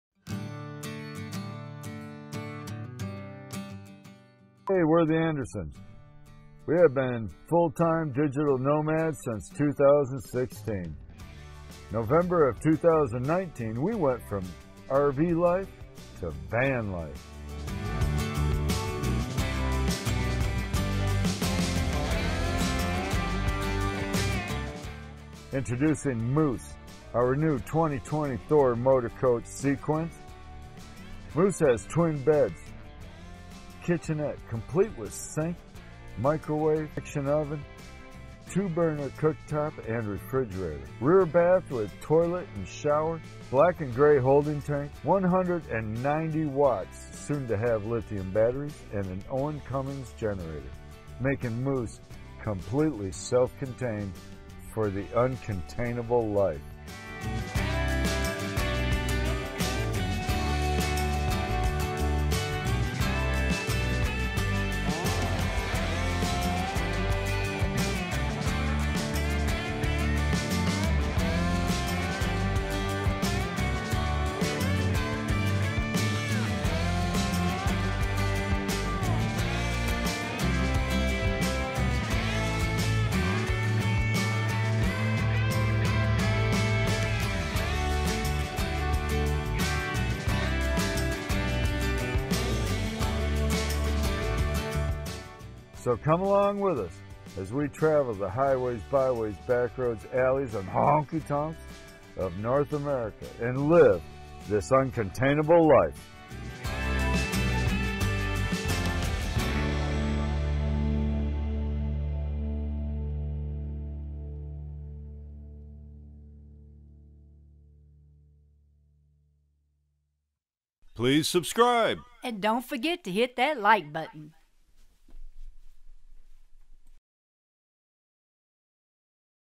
From RV Life to Van Life! Introducing MOOSE!
Introducing our new 2020 Thor Motor Coach Sequence! Name it "MOOSE"!